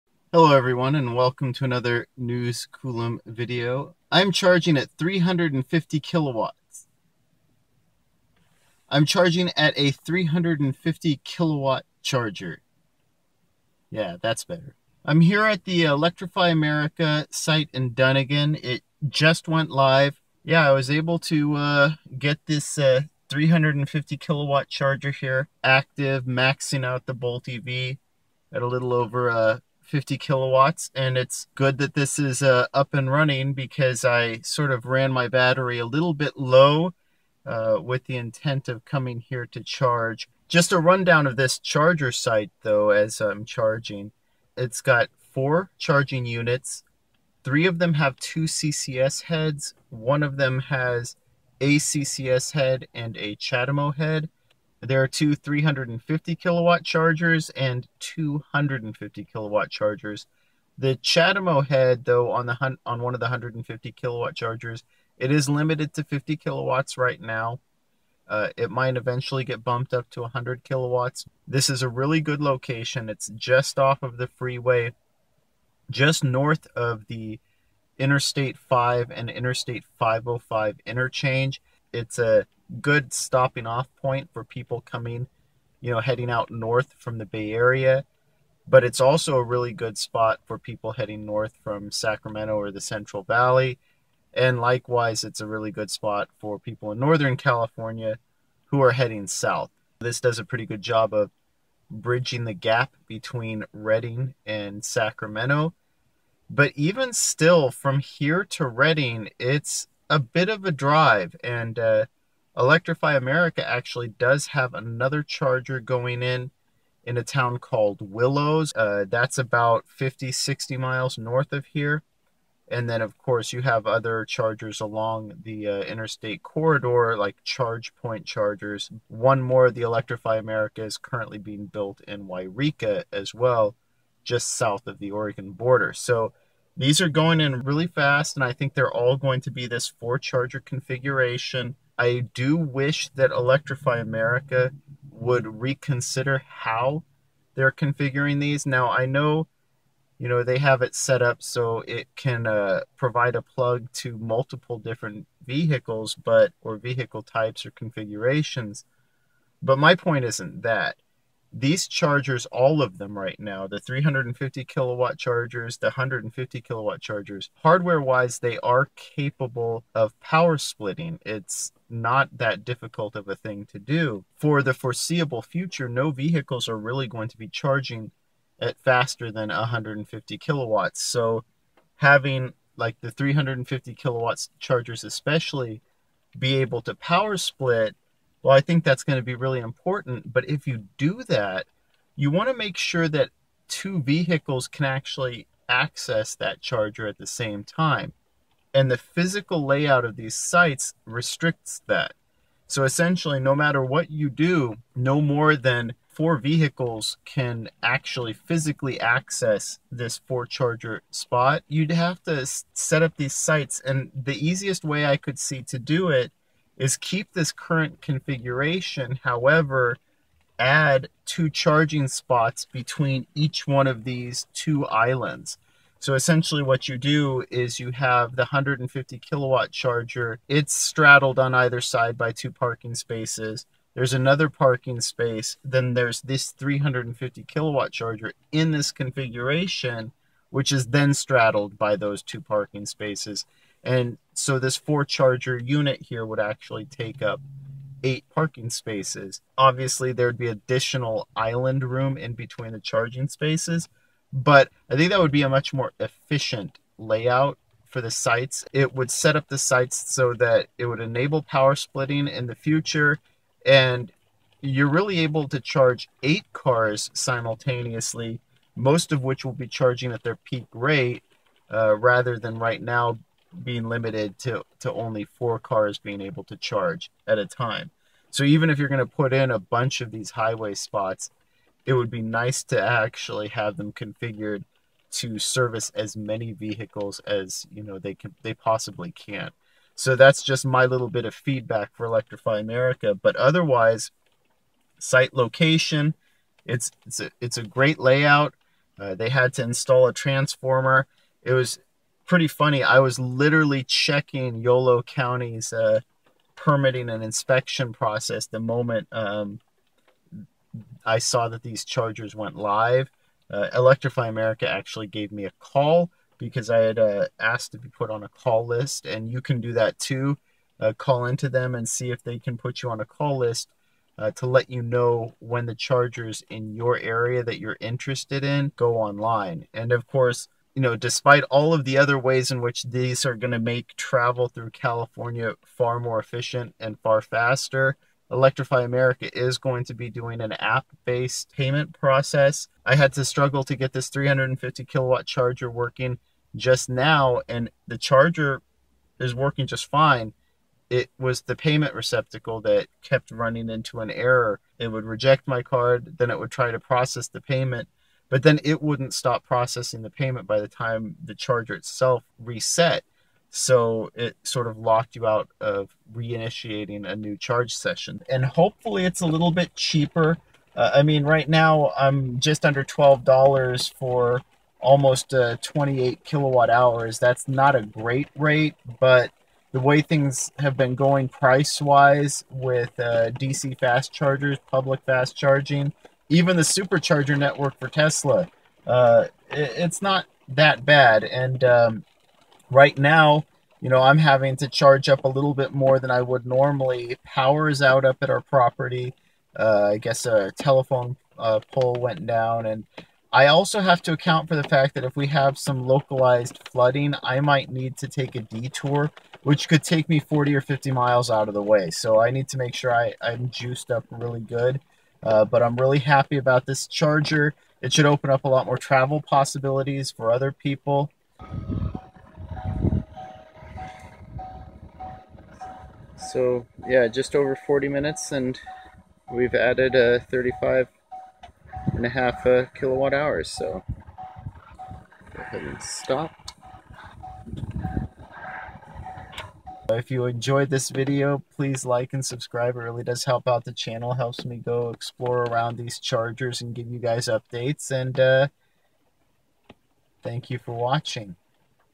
350 kW Electrify America Site at Dunnigan, CA Live
Electrify America's Dunnigan, CA site is now live. This site is at a crucial interchange between I-5 and I-505 north of Sacramento and San Francisco. The site features two 150 kW and two 350 kW DC fast chargers with CCS adapters. One of the 150 kW chargers also provides a CHAdeMO plug that is currently limited to 50 kW.